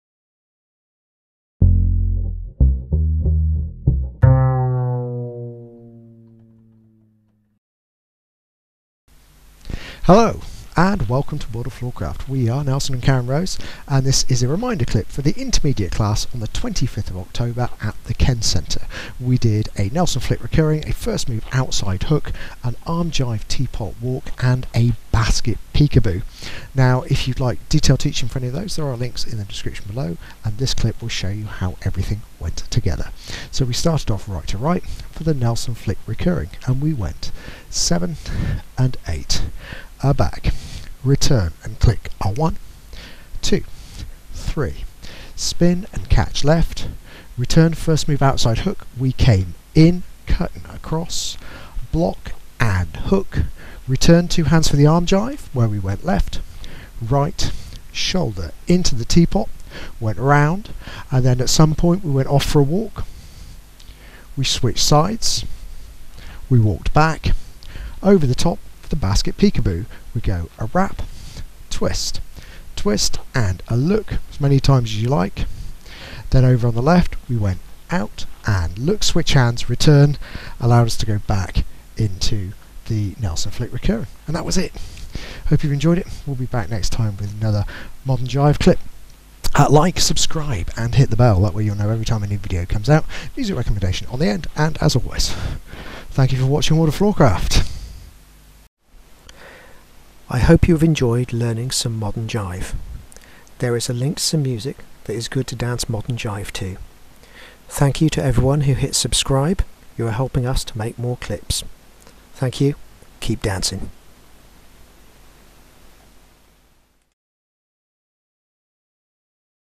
Class Reminder - Kenn Centre Intermediate 25 10 22
An Intermediate level Modern Jive video to act as a reminder of the Intermediate Class Routine at the Kenn Centre on the 25.10.22. If you need more than a reminder check out the links below which contain teaching details for the moves.

Happy dancing all.